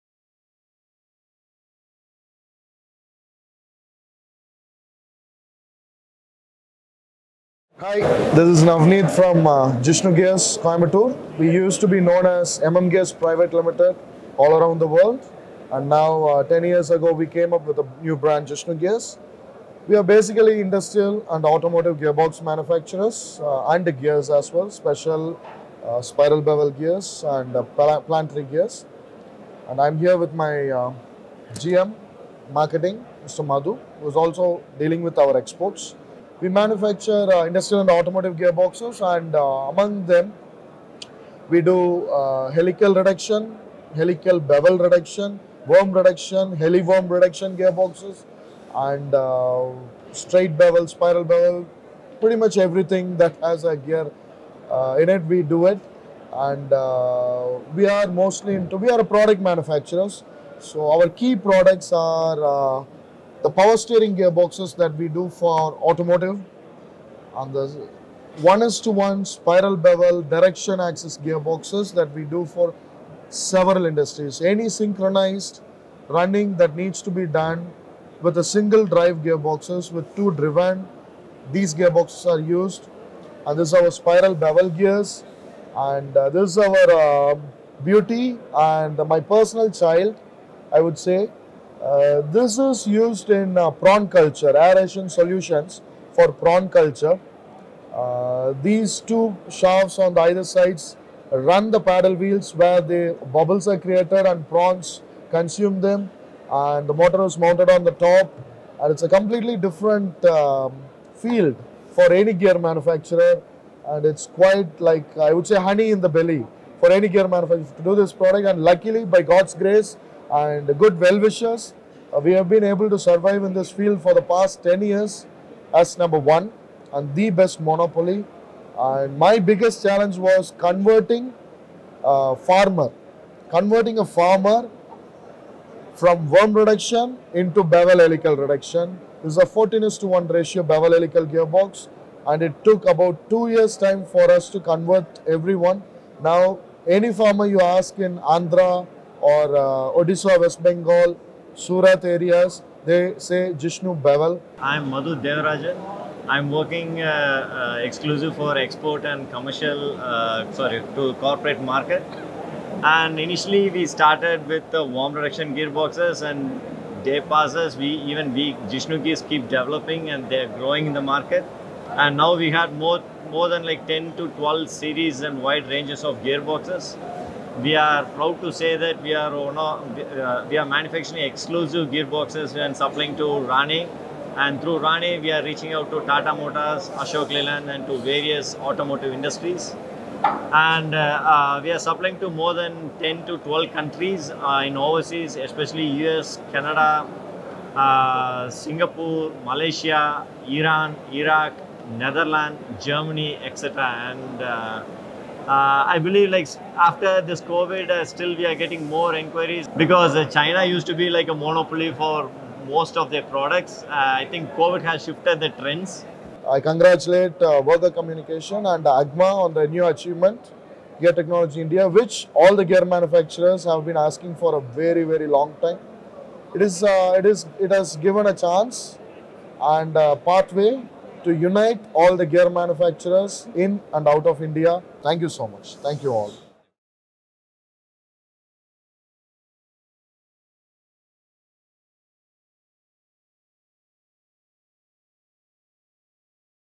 Gishnu Gears talks about their products, and why they’re looking forward to the launch of GTI
Gishnu Gears is a company that makes industry and automotive gears and gearboxes. Their expertise includes Helical Bevel Gears, Helical Worm Reduction Gearboxes, Helical reduction, Worm Reduction, Straight Bevel and Spiral Bevel. Hear from them why the launch of GTI will give a big boost to the gear industry, and contribute to new technology and innovation.